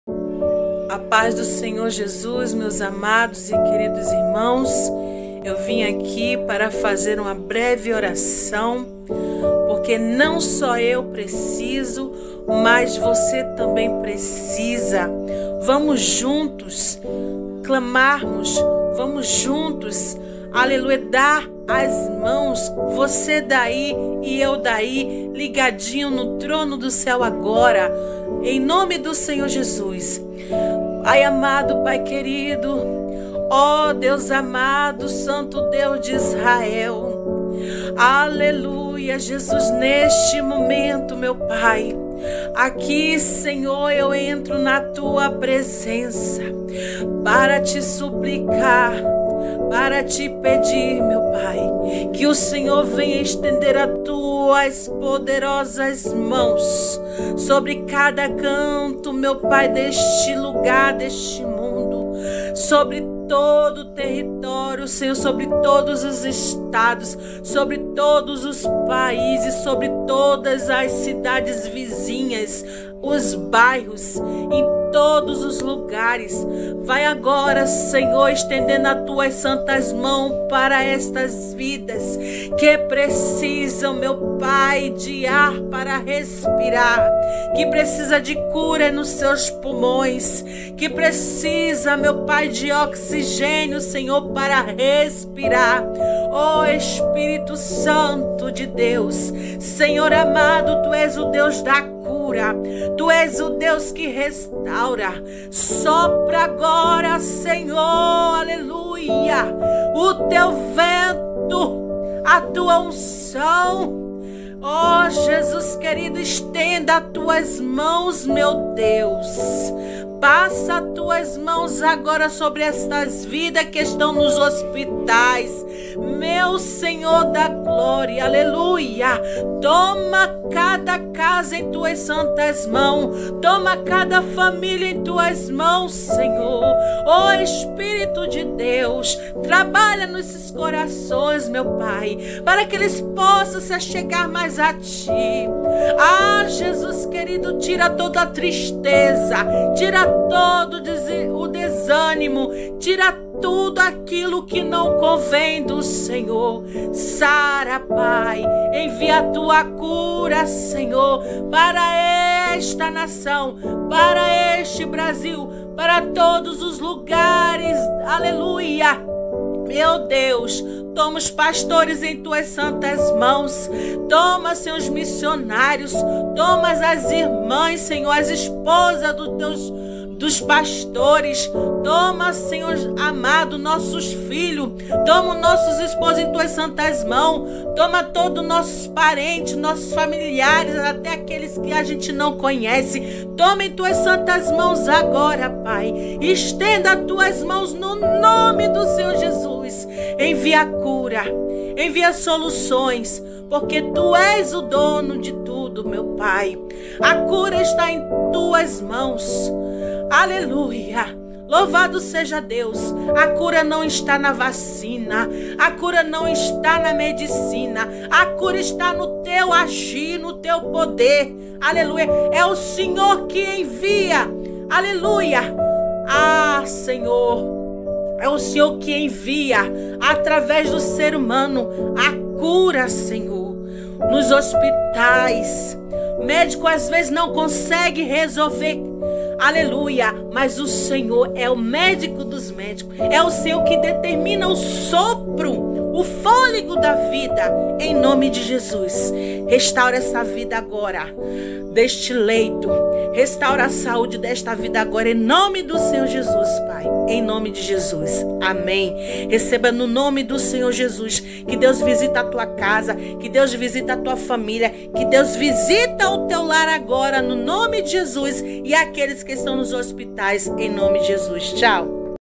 vamos juntos orar ao Senhor, você daí e eu daqui. Não importa a distância. Deus ouve
Quanto mais orarmos, mas se achegamos a Deus. Que Deus traga cura tanto espiritual como física.  vamos dar as mãos juntos em oração clamando ao Senhor Jesus